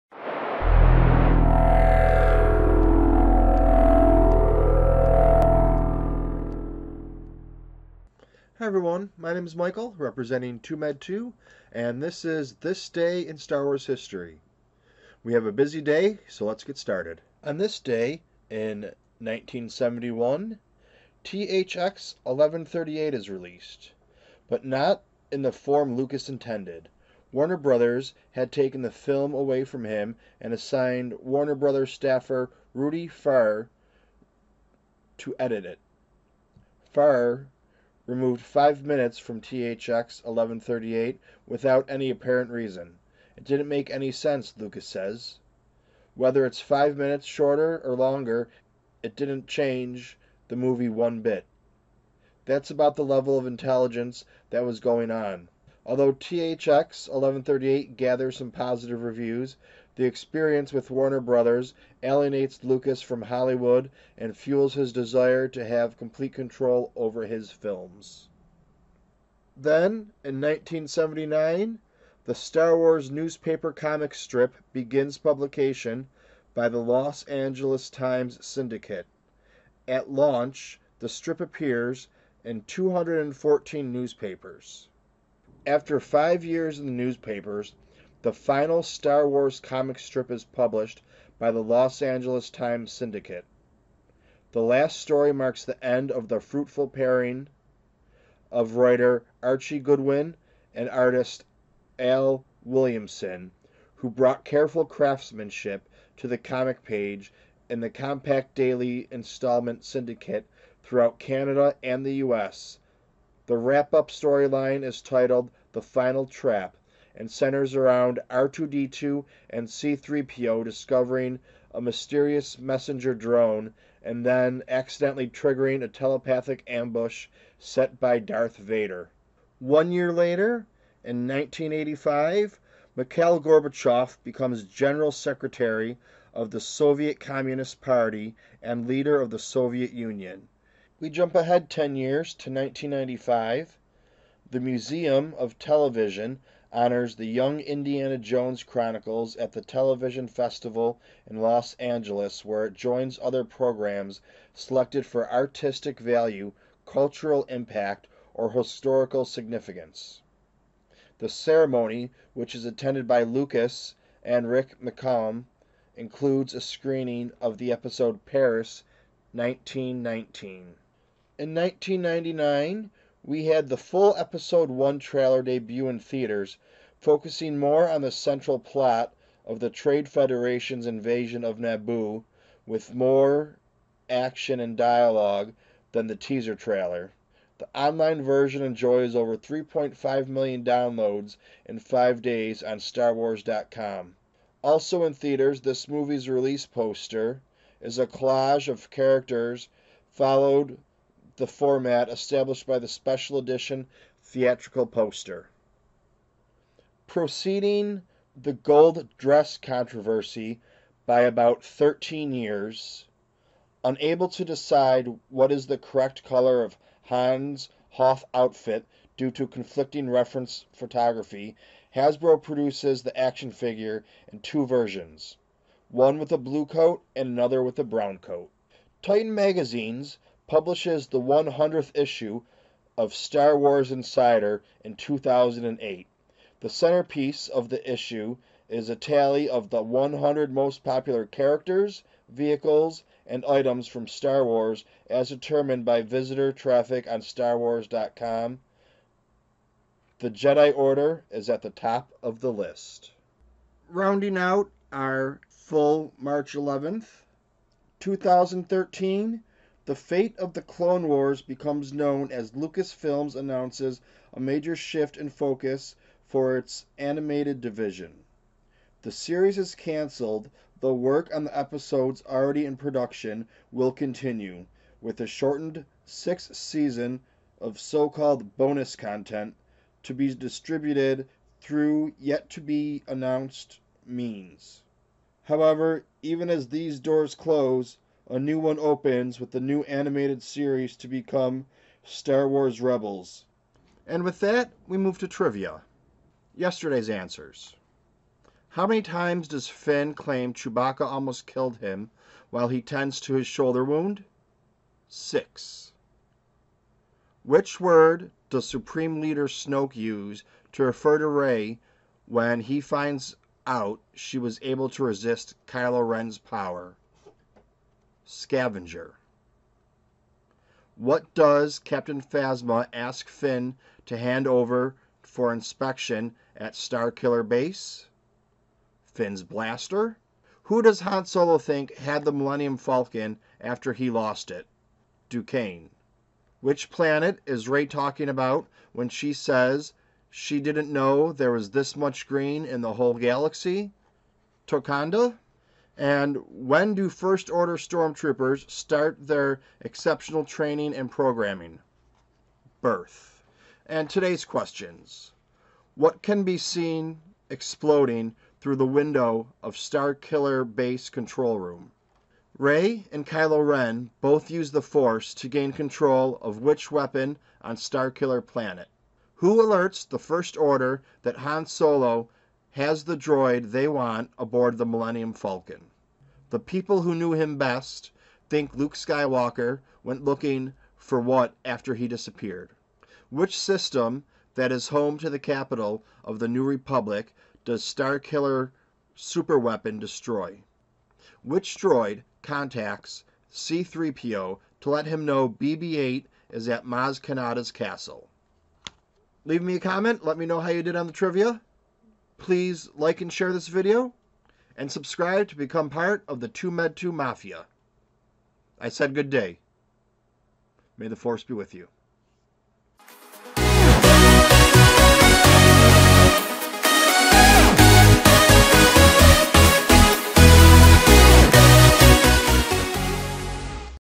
Star Wars:This Day In Star Wars History 3/11
STAR WARS: This is what happened on this day in the history of Star Wars, and Trivia Questions

       Toomedtoo

Toomedtoo.com

Tatooine Sons Info:

THE MOVEMENT

 Star Wars news podcast daily

       TatooineSons

Places to Find Restoration in Humanity